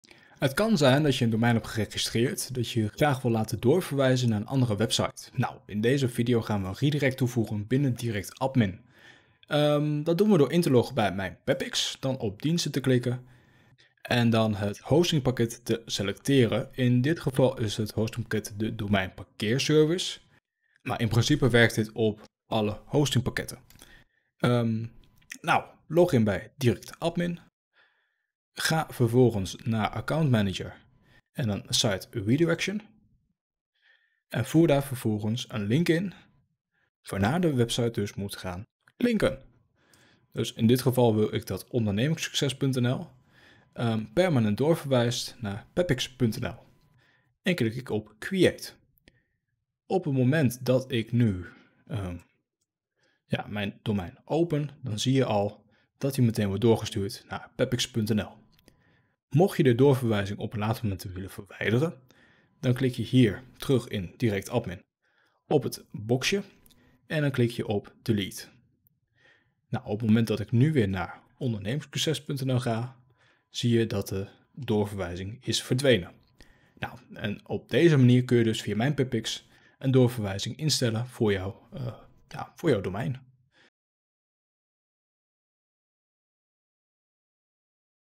Domein doorverwijzen naar een website
In deze video legt Noëll je uit hoe jij je domein doorverwijst naar een andere website met behulp van DirectAdmin.

Wil je meer leren over DirectAdmin?